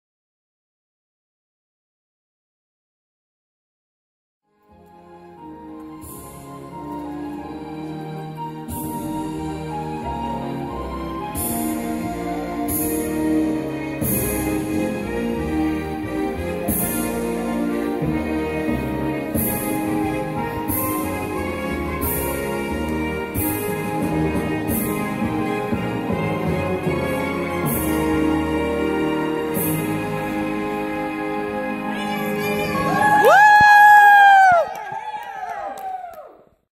landslaget2015 tränar på inmarsch inför NM 2015
När vi ändå hade publik på workshopen passade vi även på att träna på inmarschen till Nordiska Mästerskapen i Helsingfors den 5-6 december 2015.
Vi hade tyvärr inga pallplatser annars hade vi sett till att träna på det också! ;)